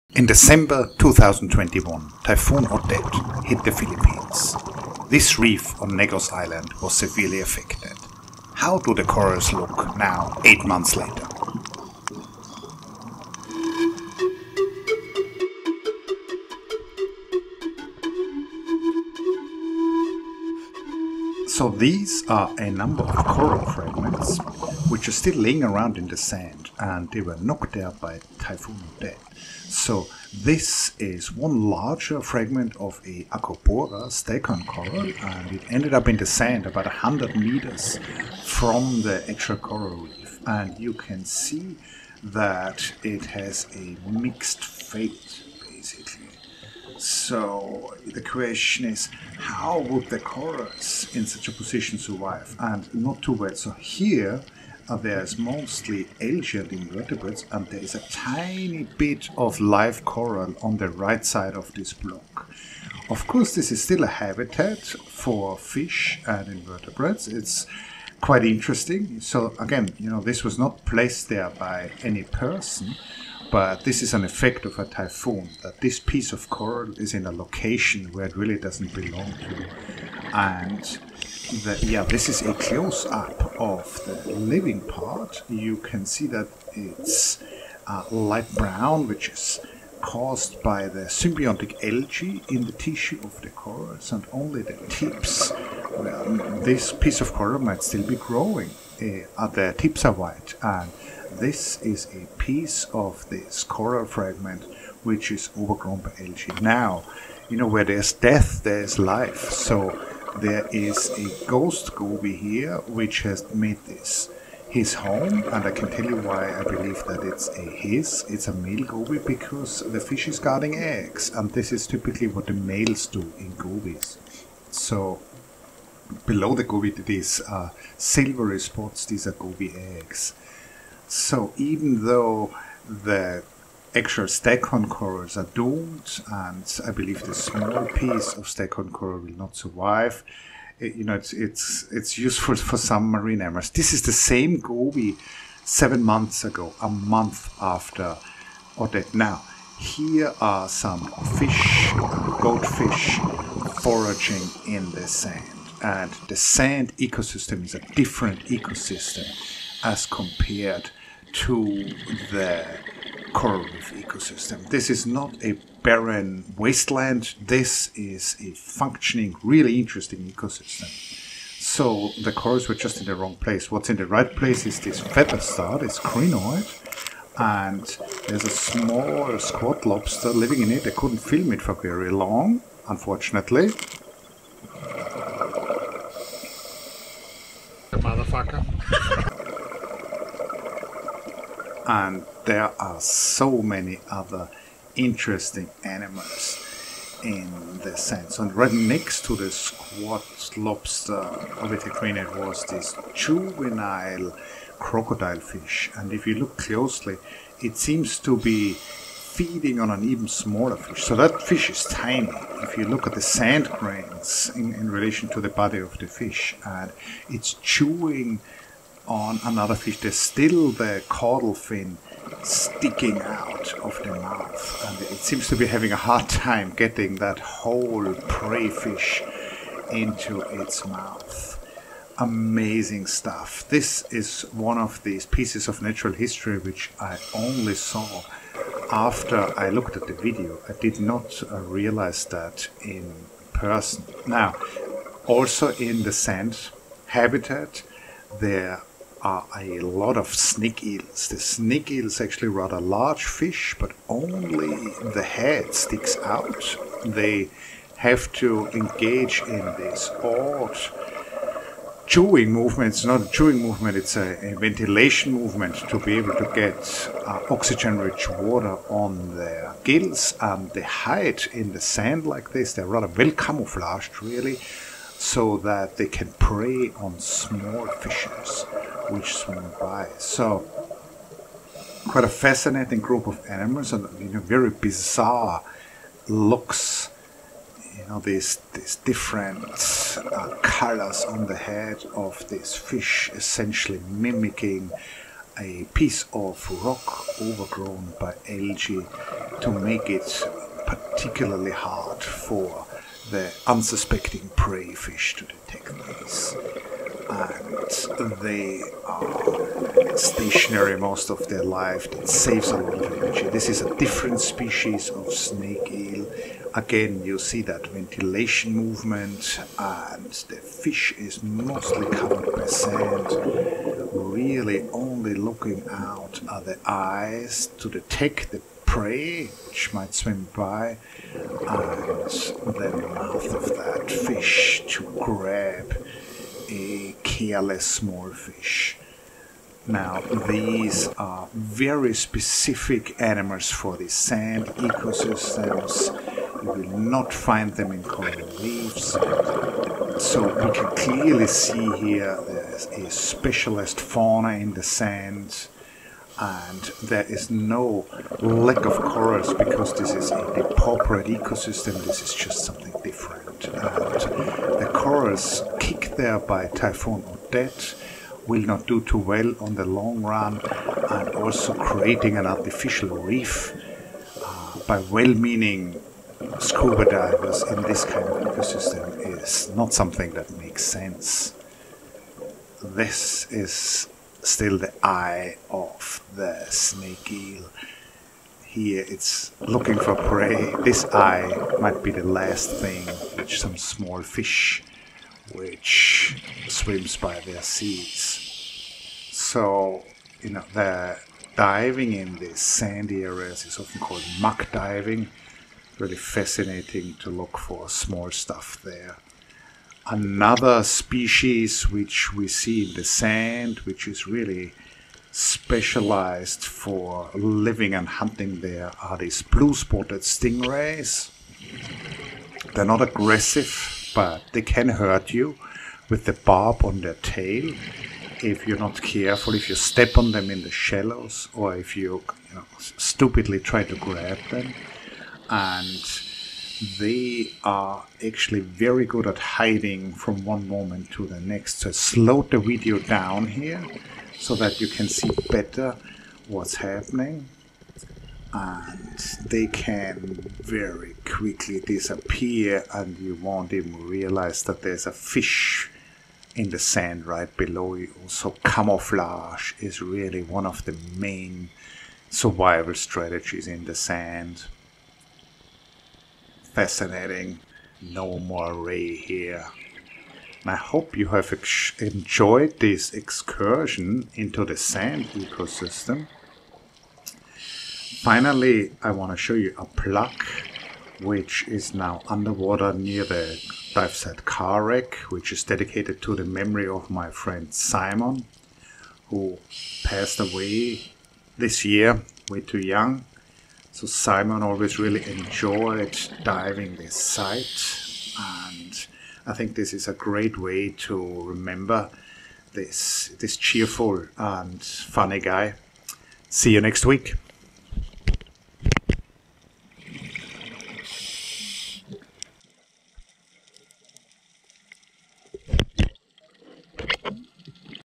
Typhoon Odette: 8 Months Later PLUS Sand Ecosystem
8 months after typhoon Odette, what happened to corals which were displaced from the reef and landed in a sandy area? Also: the sand in the tropics is home to a unique fauna, it's not an empty place devoid of corals. Also: Simon's memorial plate on the car wreck dive site in Dauin.
Intro panflute solo by Carlos Carty via Creative Commons.